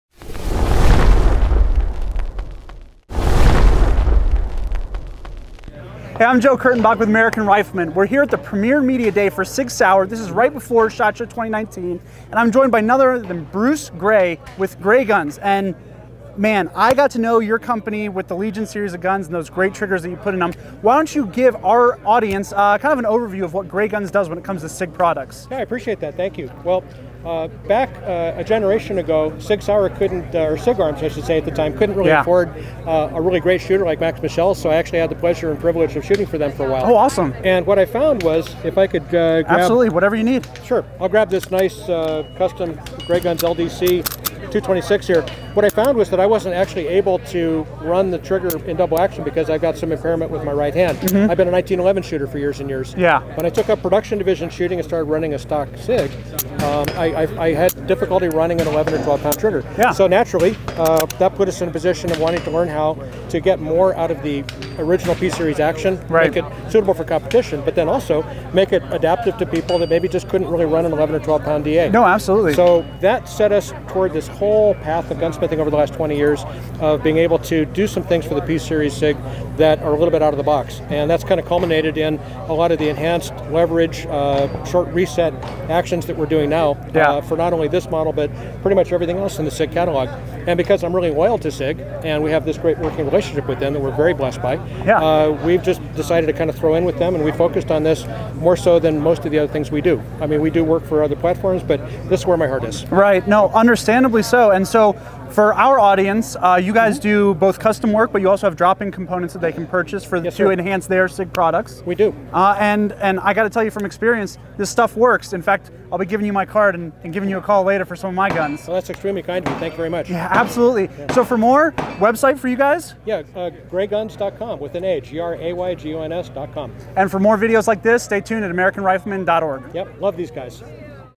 SHOT Show 2019: Gray Guns
American Rifleman's Joe Kurtenbach met up with Bruce Gray of Gray Guns to learn how he completely modified the pistol on the SIG Sauer P320 Series of pistols that we know and love today.